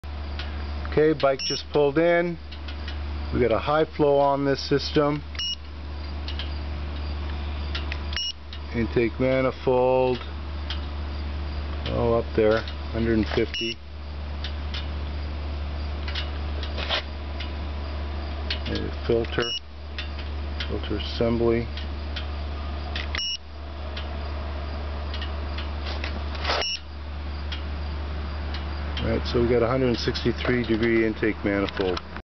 Heat test on HD high flow system
Infrared heat gun Temprature test on HD high flow system